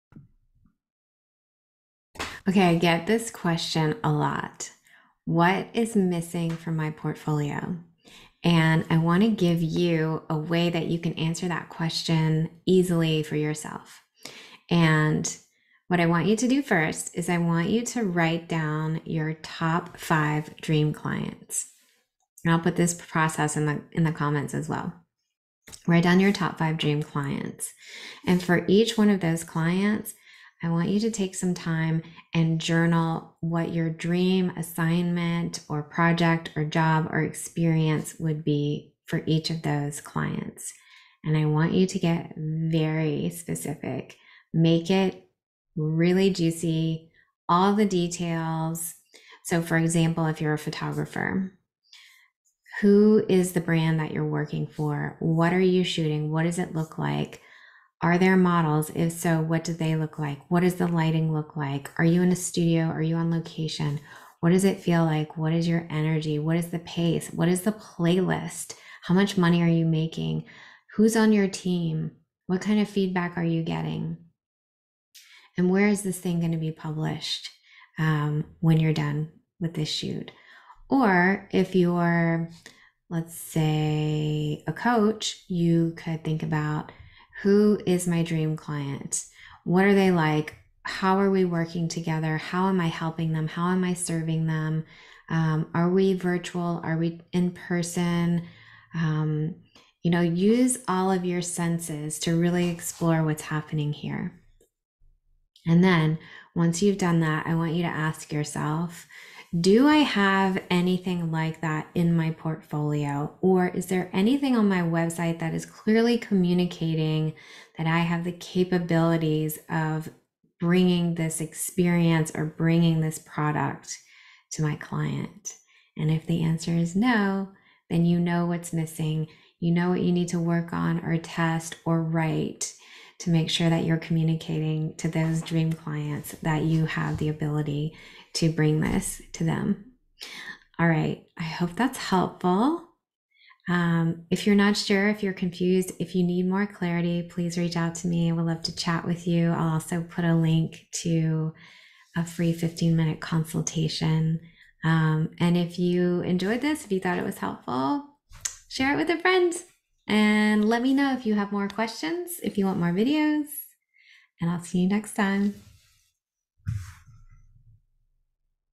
What's missing from my portfolio?
I get this question from photographers and creatives often, What's missing from my portfolio (or website)?

Here's a quick way to answer that question for yourself:

1. Write down your top 5 Dream clients.
2. For each one, journal your dream assignment/job/experience/collaboration. Get verrrrrry specific, lots of details! Use all five senses.
3. Then, ask yourself, do I have anything like this in my portfolio - or - does my website communicate that I have the experience/capability/tools/resources/offerings to make this happen?

If the answer is no, you now have a road map to what you need to add.

Let me know what you discover, or tell me, who are your dream clients!?

Need help? Hop over to my website to schedule a free consultation call.